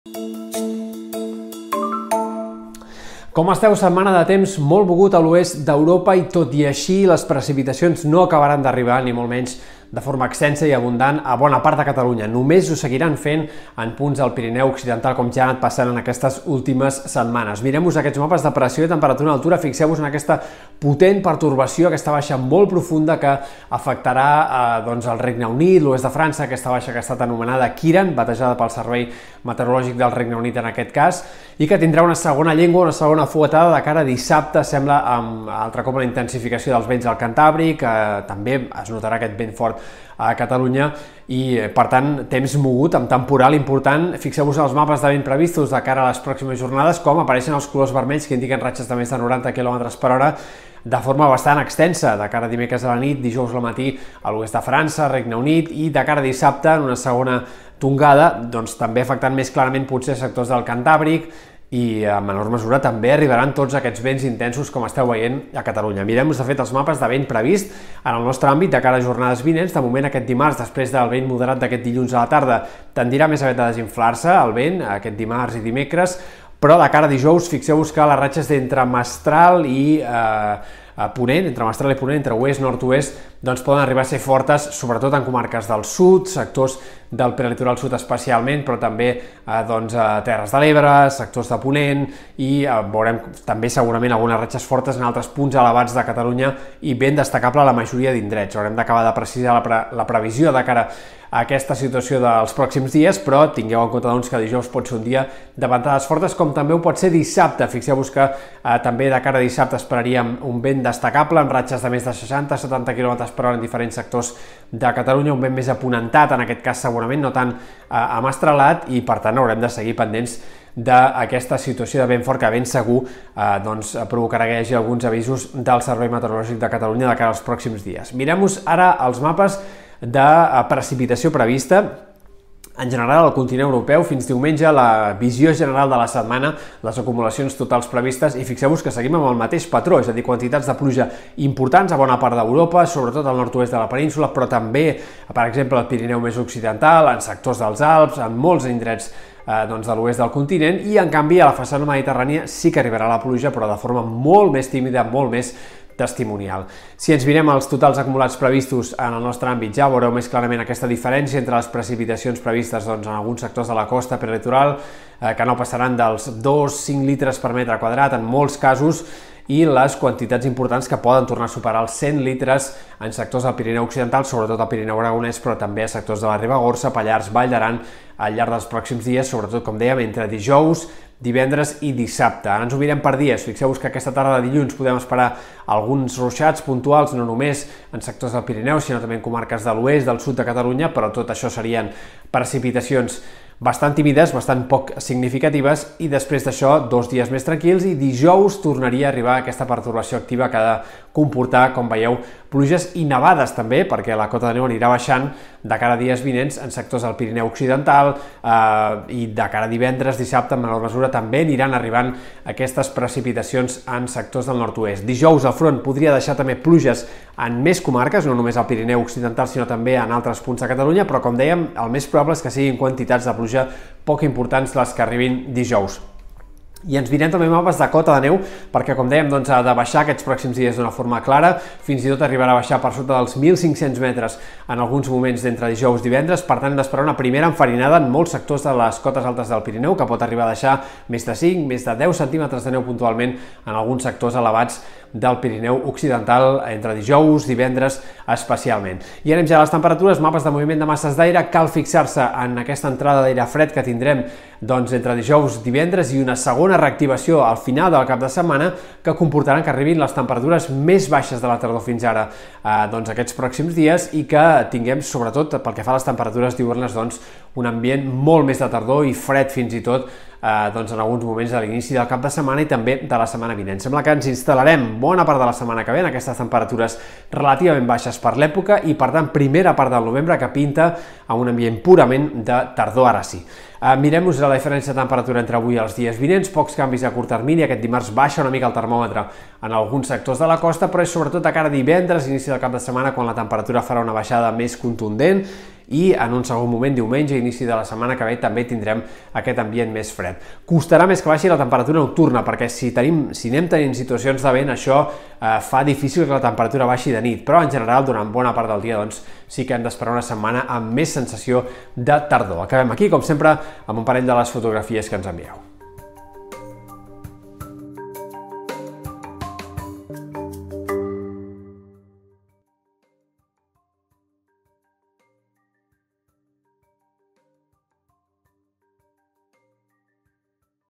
La méteo: primera visita del fred en una altra setmana amb ventades
Dos nous girs de temps entre dijous i dissabte aportaran més pluges abundants a la conca de l'Ebre.